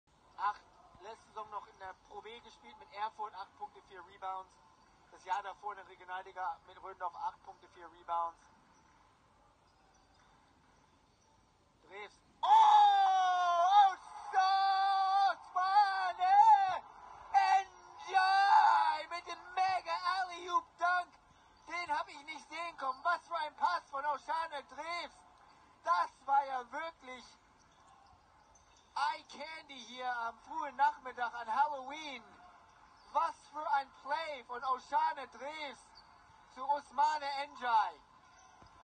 Senegal Sensation Ousmane Ndiaye Alley-oop dunk
Miles Schmidt-Scheuber(eurobasket.com / germanhoops.com) has the call as Dragons Rhondorf guard Oshane Drews serves up perfect alley-op pass to 17 year old Senegal NBA prospect Ousmane Ndiaye for the hard throw down on Halloween 2021